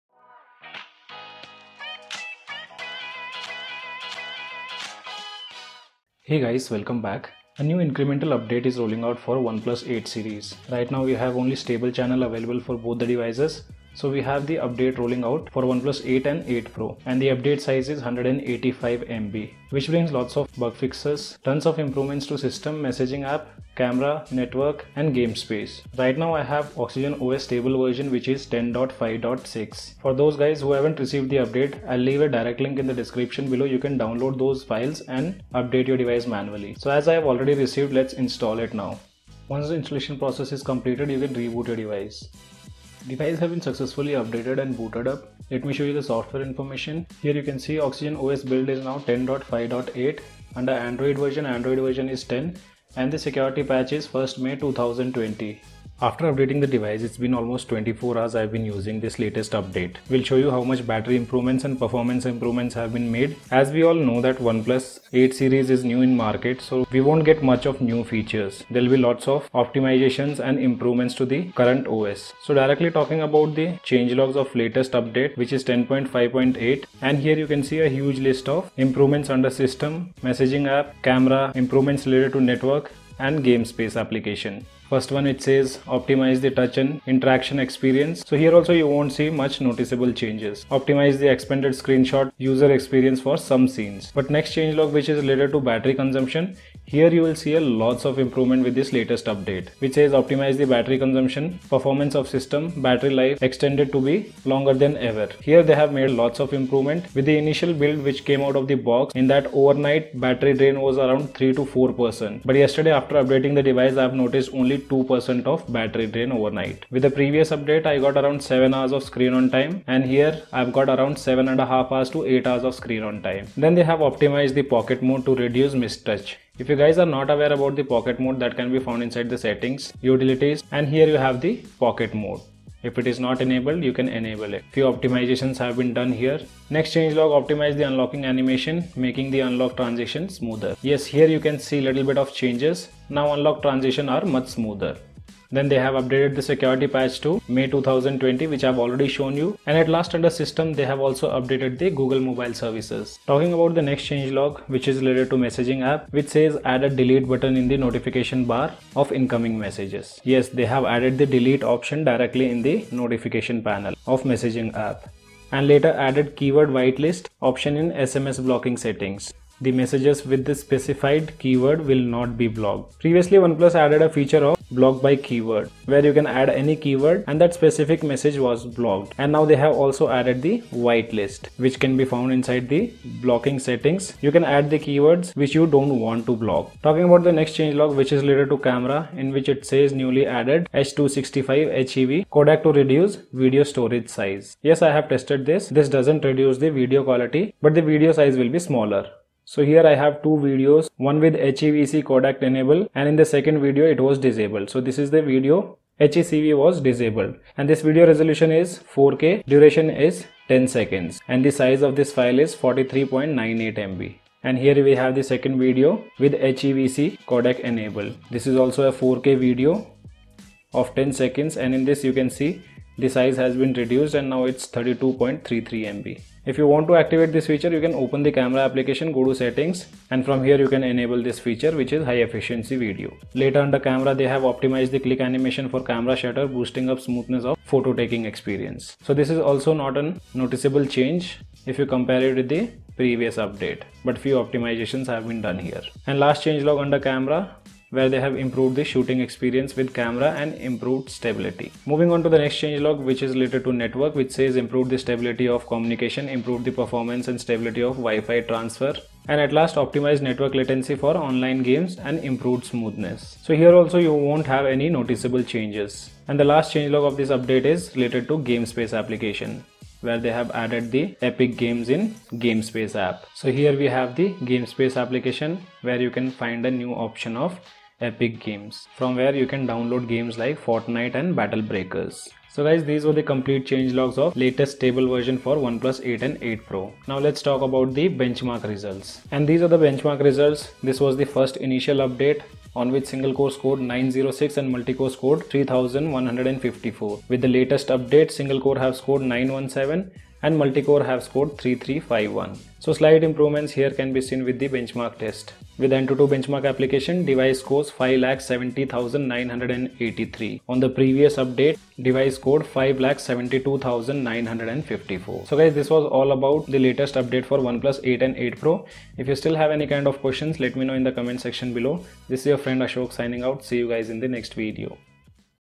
Oxygen OS 10.5.8 brings major battery improvement & bug fixes for Oneplus 8\8 Pro
Oneplus 8 series gets new Oxygen OS updates with lots of system,camera and network optimizations.The latest update has also brought May 2020 Android security patch.